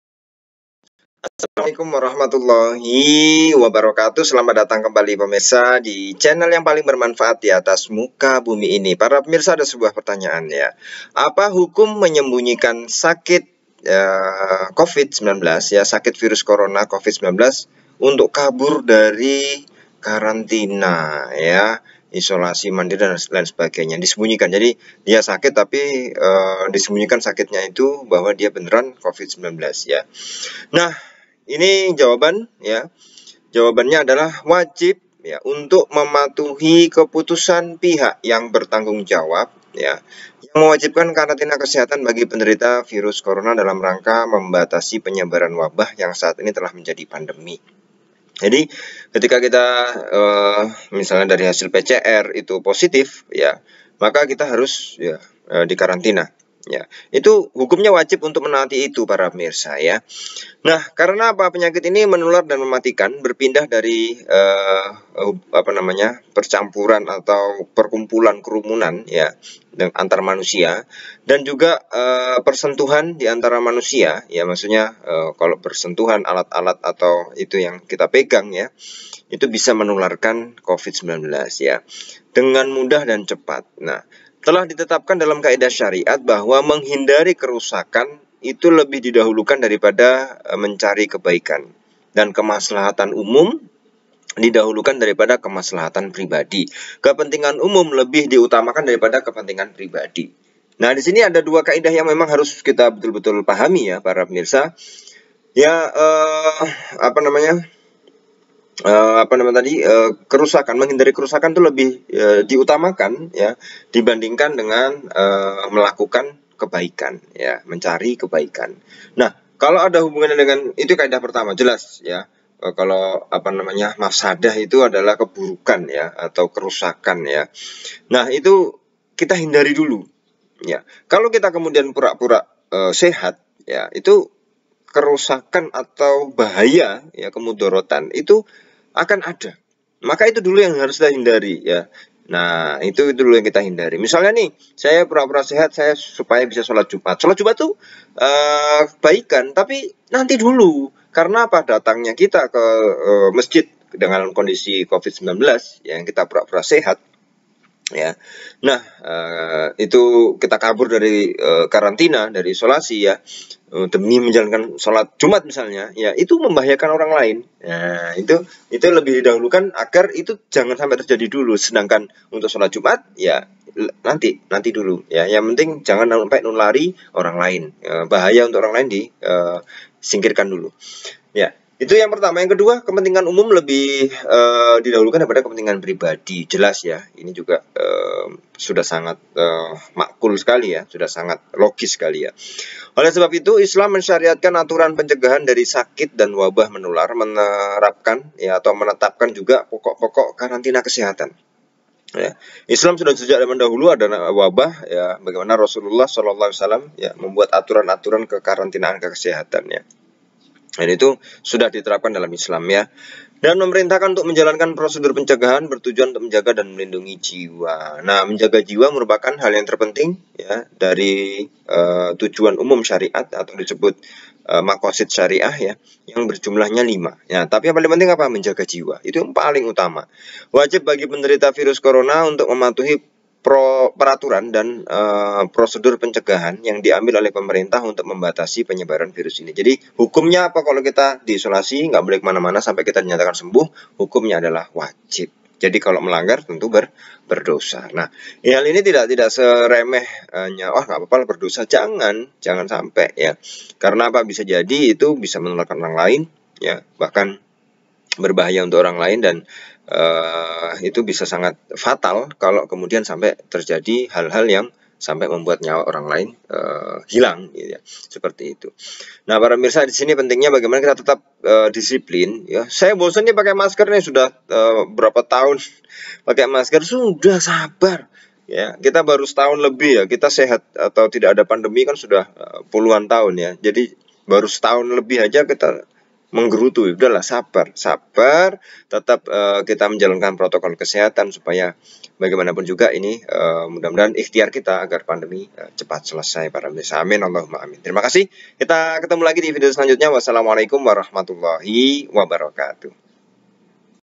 Positif Covid-19 Tapi Kabur Dari Karantina dan Isolasi, Bagaimana hukumnya dalam Islam?
Mudah2an bermanfaat...